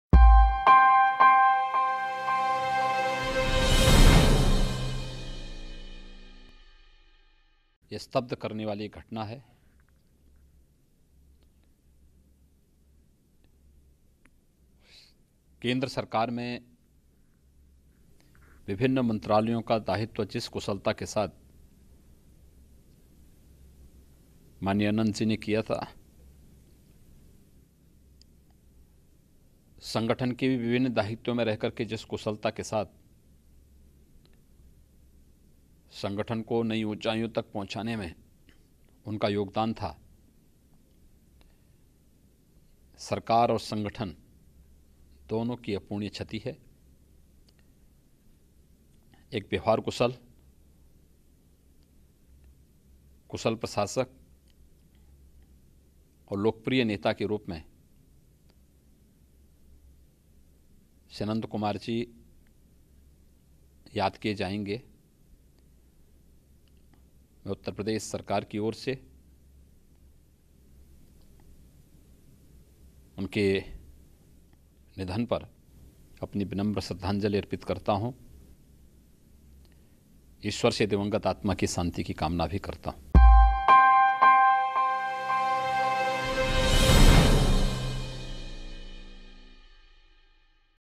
CM Yogi ने Ananth Kumar के देहांत पर जताया दुख
Uttar Pradesh Chief Minister Yogi Adityanath expressed his heartfelt condolences over the demise of Union Minister Ananth Kumar.